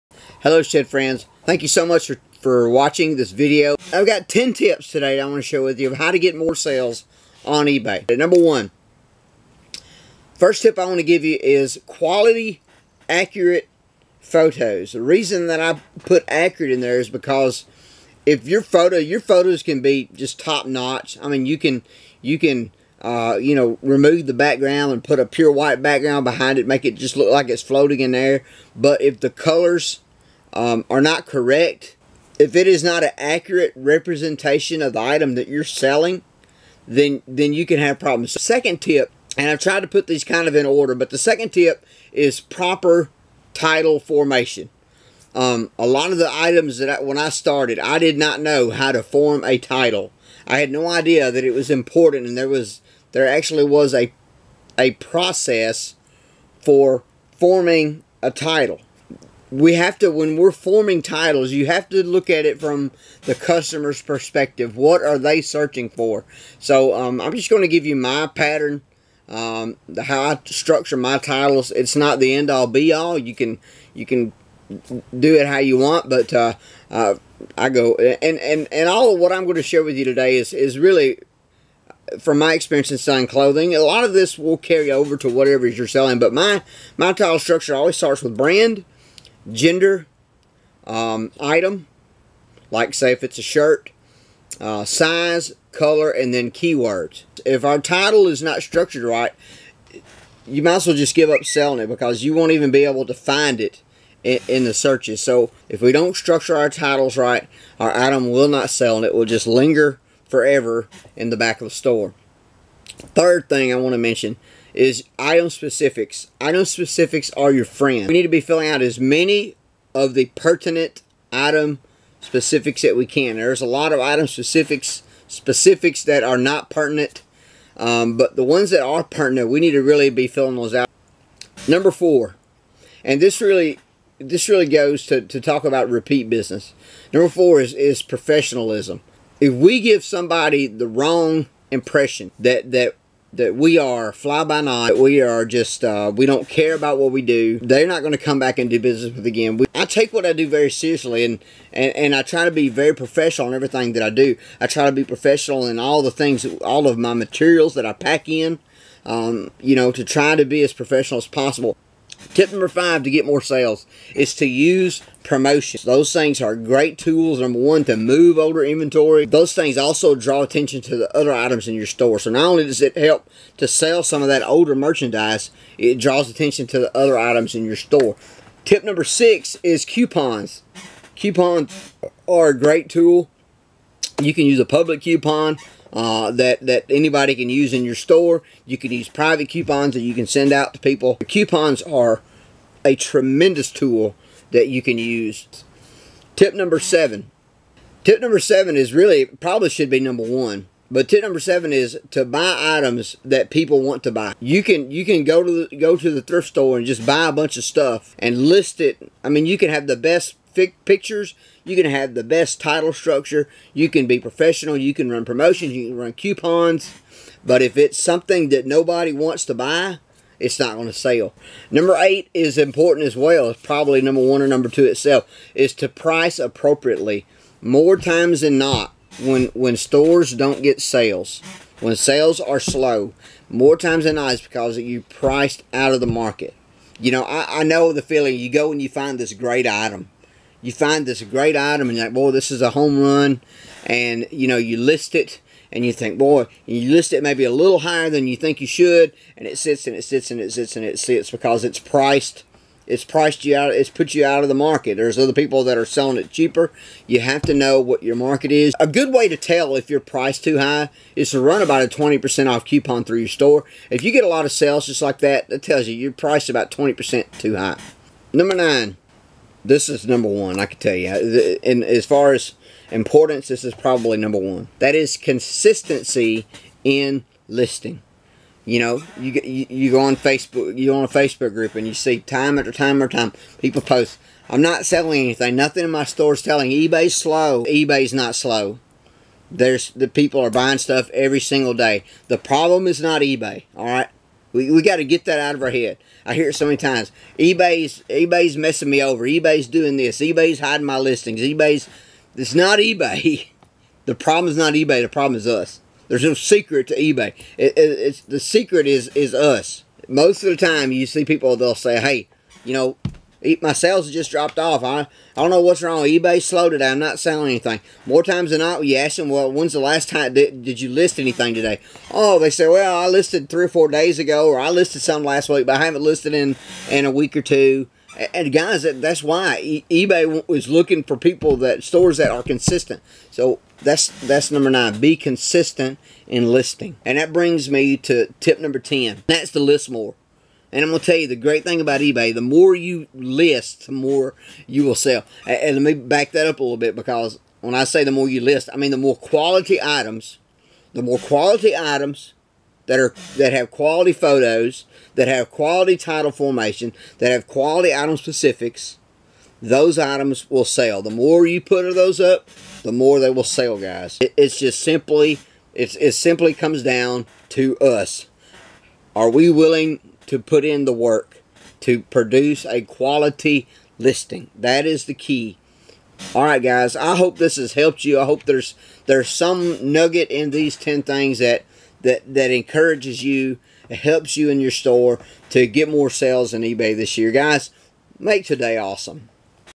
10 Tips To Have More Sales On Ebay
10 tips to having more sales on EBay

1.Quality Accurate Photos
2. Proper title formation
3. Item specifics are your friends
4. Professionalism
5. Promotions
7. Buying Items customers want to buy
8. Price appropriately
9. Consistency
10. List More